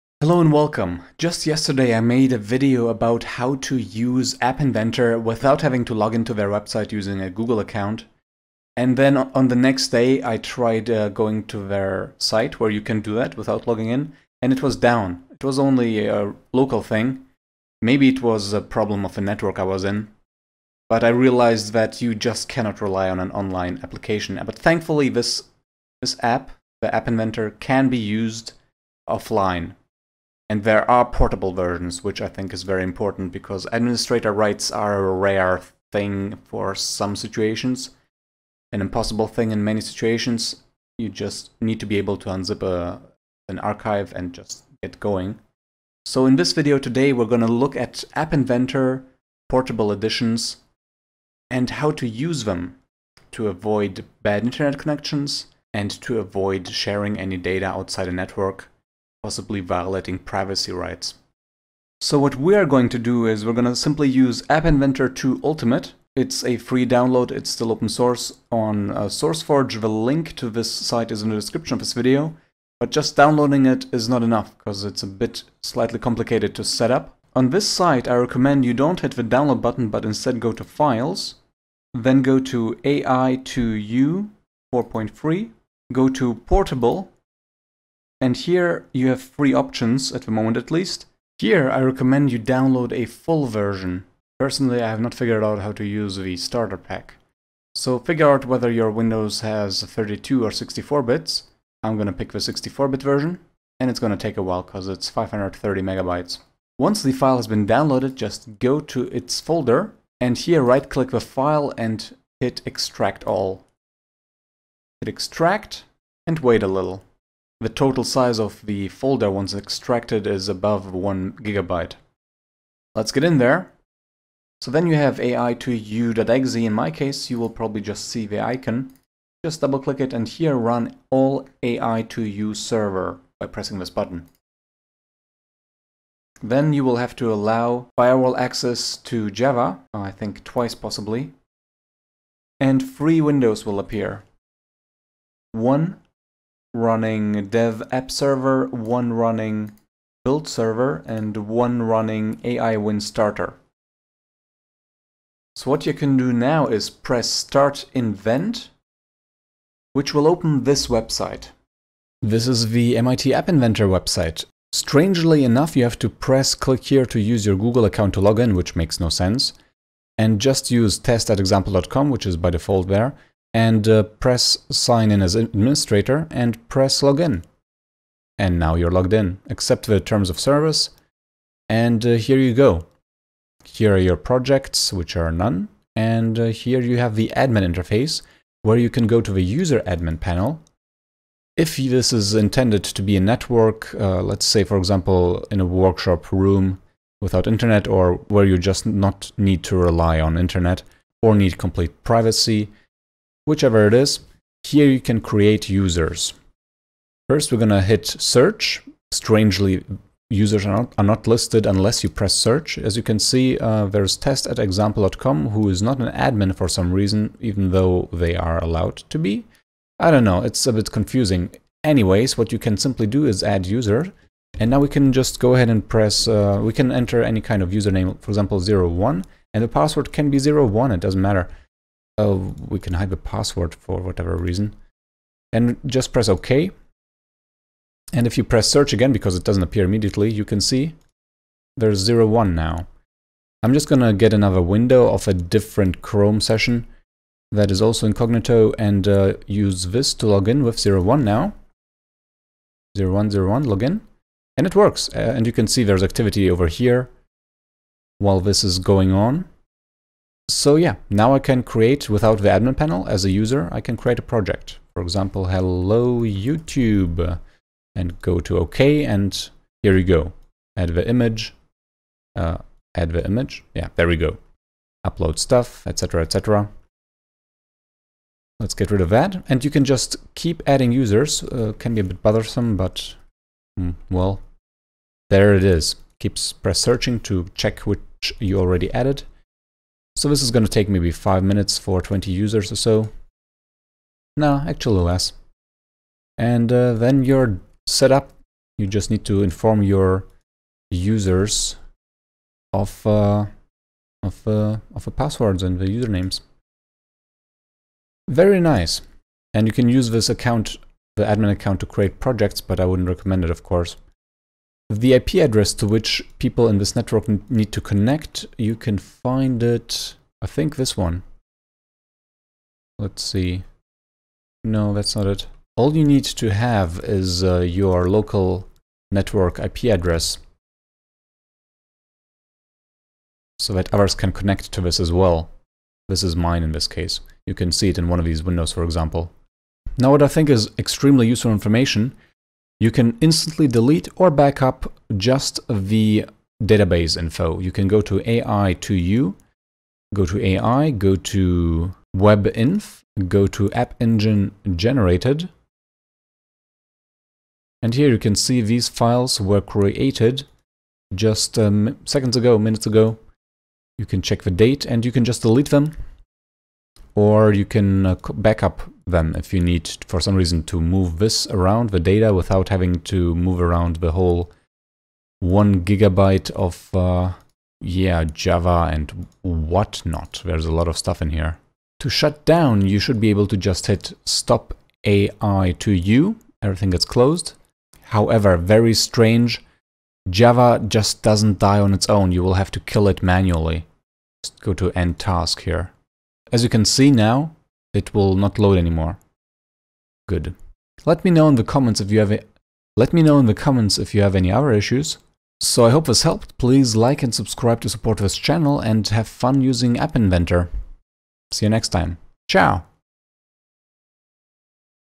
App Inventor 2 Offline Portable - How to Download, Install, Setup, Use
How to use MIT AppInventor 2 without having to be connected to the internet or logging with a google account or suffering from slow speed and possibly data loss from unstable WiFi.

Download the portable version AI2U for free - THANK YOU, DEVELOPERS!

There is an official offline/Windows server version but seems to be extremely non-portable unfortunately (can only run if in C:\Program Files (x86)/ ?